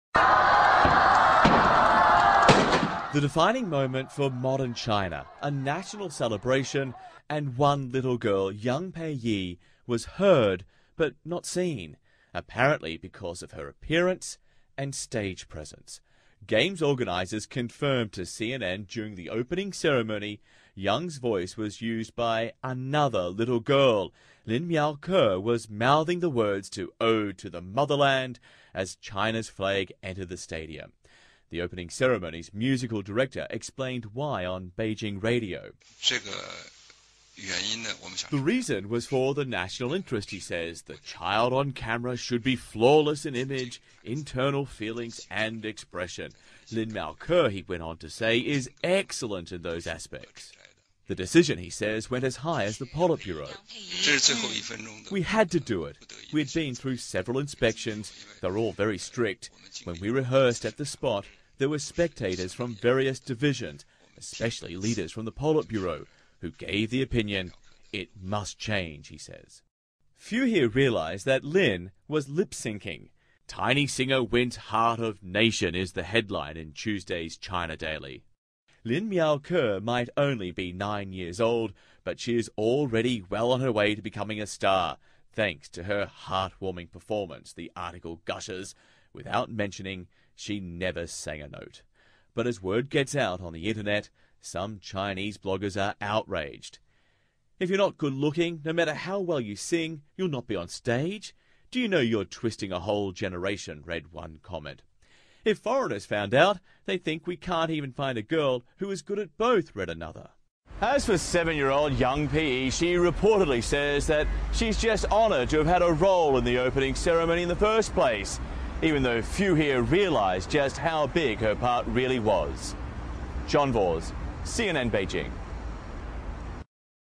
Beijing Fakes the Olympics
One had the voice and one had the looks. Performed in front of billions of people on TV, Lin Miaoke, Aged 9, lipsync'd on the Beijing Opening Ceremony and impressed millions and millions of people. She was promised with a promising carreer in the future but little did we know, she actually mouthed the words during the night.

Behind the performance there was infact another young girl aged 7 who actually sang the song but was not given credit to. She didn't perform on the stage because she wasn't "cute enough".

"We had to do," the music director said, "the reason was for the national interest, the child on camera should have flawless image and expression." He said," Lin Miaoke  was excellent in all of those areas."

Chinese bloggers backup the situation, "If you're not good looking enough, no matter how well you sing, you will not be on stage, do you know you're twisting a whole generation? If foreigners found out, they'd think we can't even find a girl who is good at both."

Little amazing girl, Yang Peiyi says she's just honoured to be part of the ceremony.

Oh yea! Forgot to mention, China also faked the fireworks! Haha. I'm so ashamed of China. At first I was so proud of the country where my ancestors were from, but now, im totally ashamed of China.

Do you remember the 29 Footsteps or something? Well that wasn't live. They recorded that digitally because they were afriad they wouldn't be able to capture it live.

I would like to see that young girl Yang Peiyi sing at the closing ceremony, if you feel the same, please sign this petition.